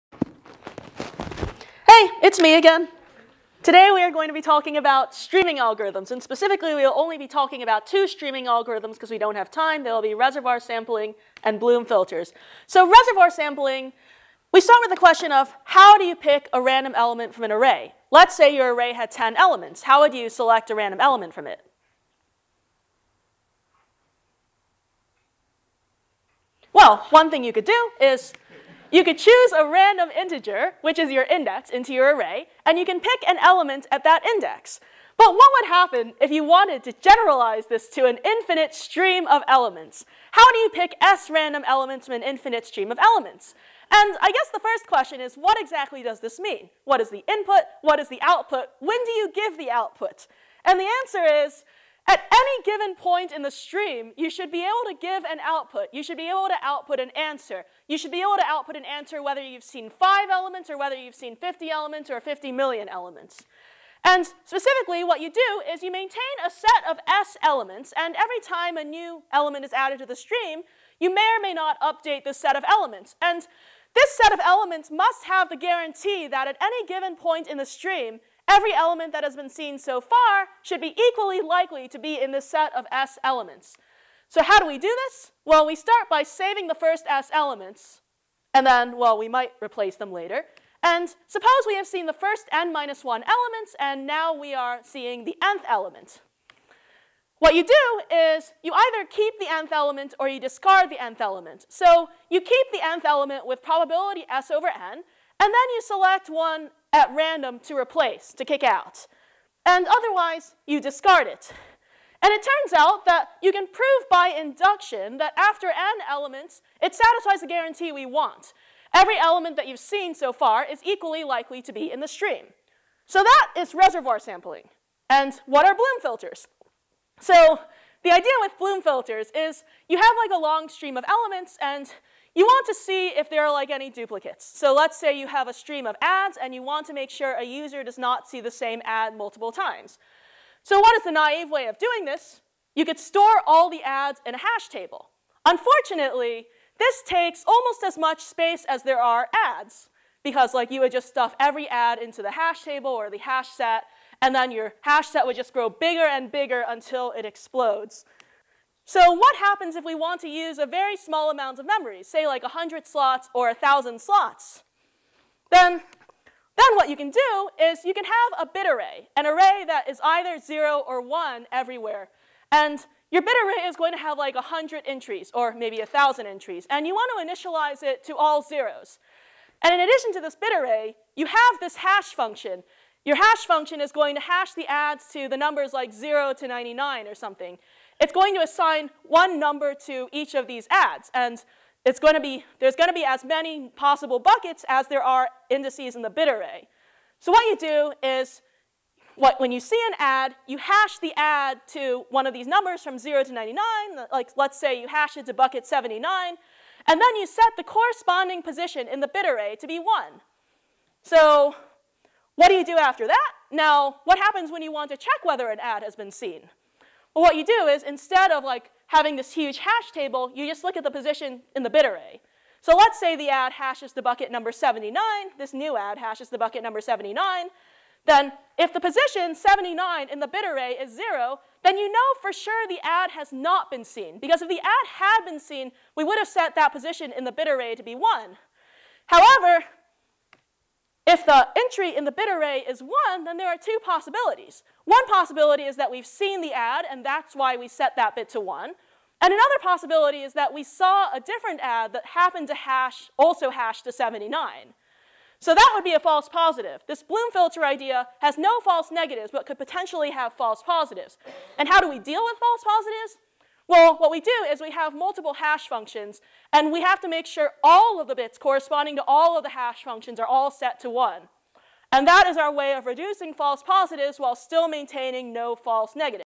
Streaming Algorithms (Jessica Su)
A brief explanation of reservoir sampling and Bloom filters.